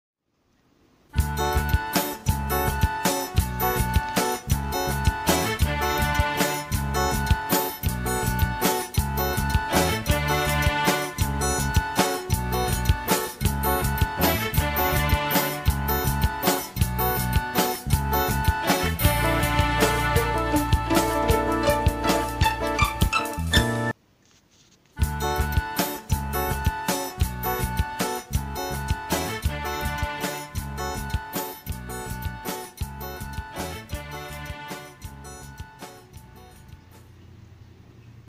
Huawei P30 pro VS Huawei P10 - GPS
Un aperçu de la rapidité de réception du GPS de Huawei P30 pro, rapide, précis et fidèle c'est parmi le meilleur des GPS intégrés à un smartphone. Il capte les réseaux GPS (USA), Glonass (Russie), Beidou (Chine), Galiléo (Europe), QZSS (Japon) et IRNSS (Inde).